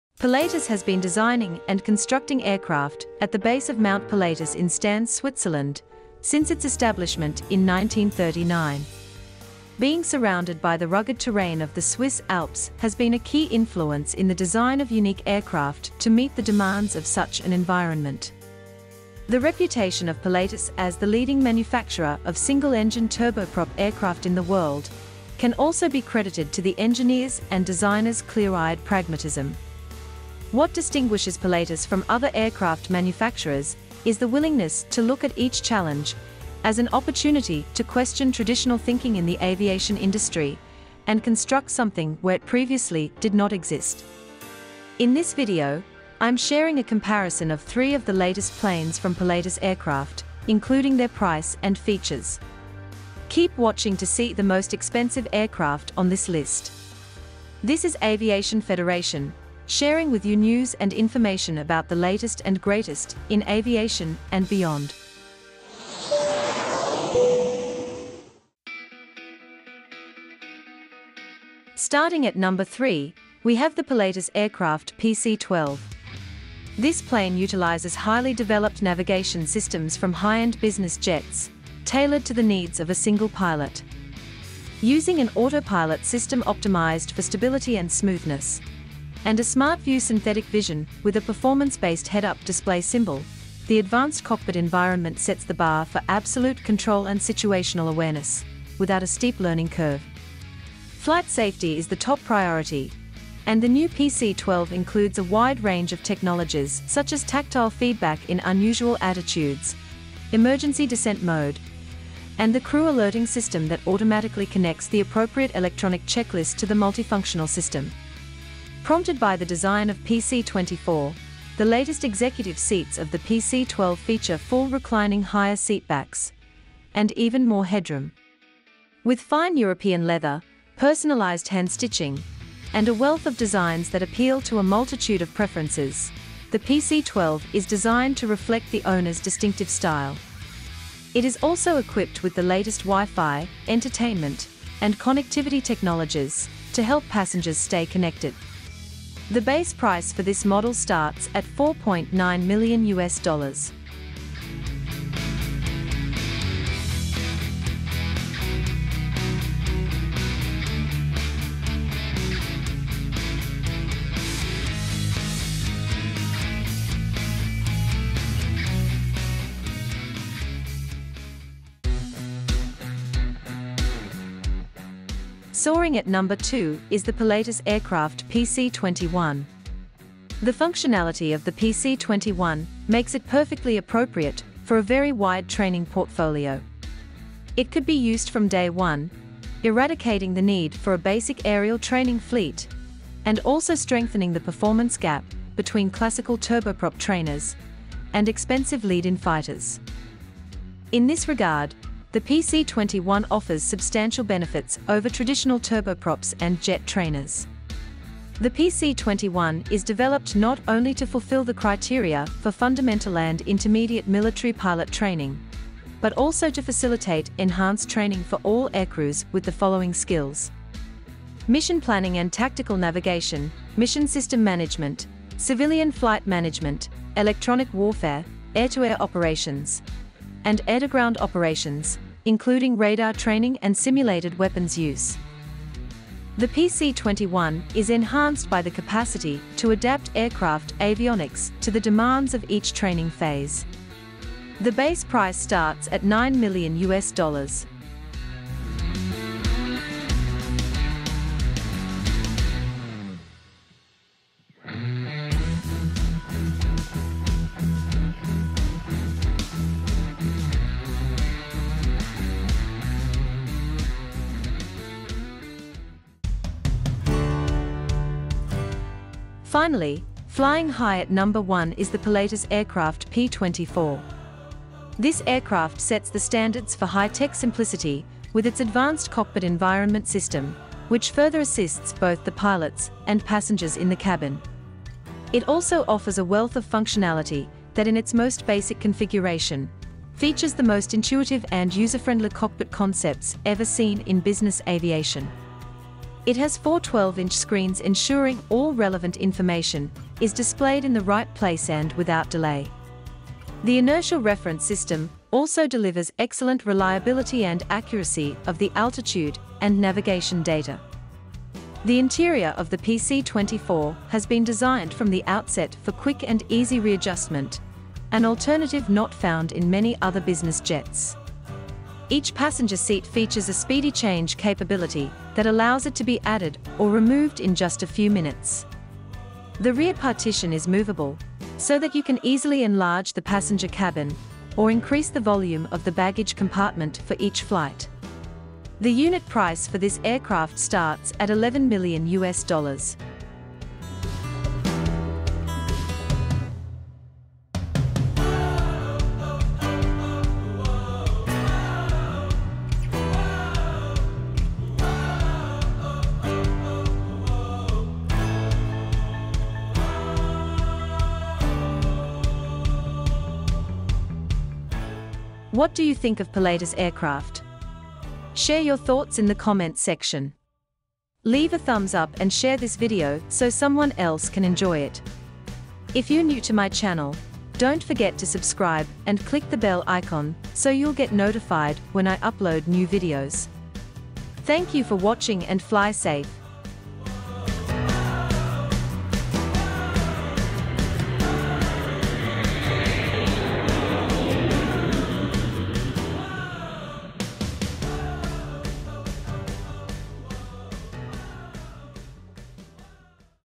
Top 3 Pilatus Aircraft Comparison | Price & Specs
In this video, we're sharing a comparison of three of the latest planes from Pilatus Aircraft, including their price and features. Stay until the end to see the most expensive aircraft on our list.

Pilatus has been designing and constructing aircraft at the base of Mount Pilatus in Stans, Switzerland, since its establishment in 1939. Being surrounded by the rugged terrain of the Swiss Alps has been a key influence in the design of unique aircraft to meet the demands of such an environment. The reputation of Pilatus as the leading manufacturer of single-engine turboprop aircraft in the world can also be credited to the engineers' and designers' clear-eyed pragmatism. What distinguishes Pilatus from other aircraft manufacturers is the willingness to look at each challenge as an opportunity to question traditional thinking in the aviation industry and construct something where it previously did not exist.

Max Speed: 290 knots (537 km/h)
Length: 45.93 ft (14 m)
Wingspan: 52.49 ft (16 m)
Max Range: 1,803 nm (3,339 km)
Max Passengers: 9
Price: starts at US$ 4.9 Million

Pilatus Aircraft PC-21

Max Speed: 370 knots (685km/h)
Length: 49.07 ft (14. 95 m)
Wingspan: 57.22 ft (17.44 m)
Max Range: 1,700 nm (3,148 km)
Max Passengers: 9
Price: starts at US$ 9 Million

Pilatus Aircraft PC-24

Max Speed: 440 knots (815 km/h)
Length: 33 ft (10 m)
Wingspan: 40 ft (12 m)
Max Passengers: 4
Max Range: 2,000 nm (3,704 km)
Price: starts at US$ 11 Million

The views expressed in our videos are our opinions and may not necessarily reflect those of the companies we feature.